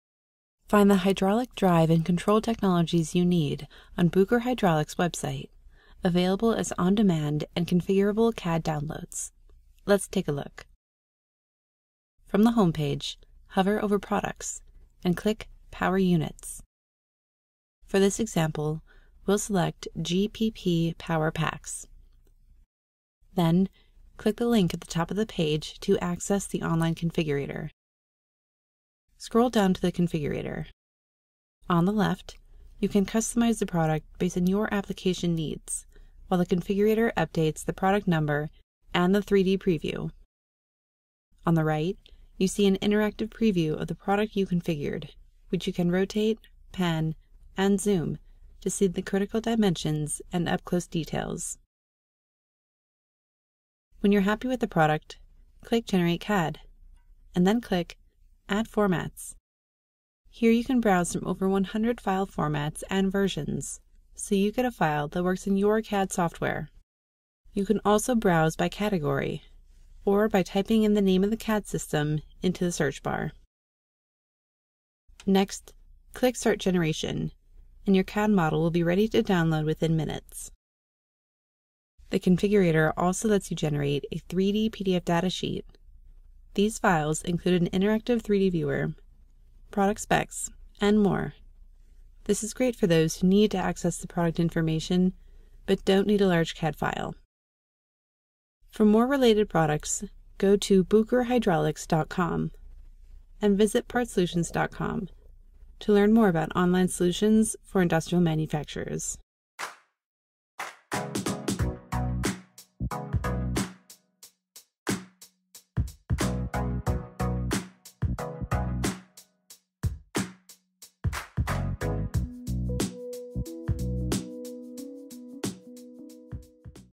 Hydraulic PowerPack CAD Models Added to Bucher Hydraulics' Configurator
Bucher Hydraulics, a global provider of hydraulic solutions for the mobile and industrial markets, has recently launched an online model builder, created by CADENAS PARTsolutions. The tool provides on-demand product configuration and CAD downloads for Bucher’s self-contained hydraulic powerpack system. This new capability enhances the online digital customer experience by enabling customers to instantly find, configure, and download product data for immediate use.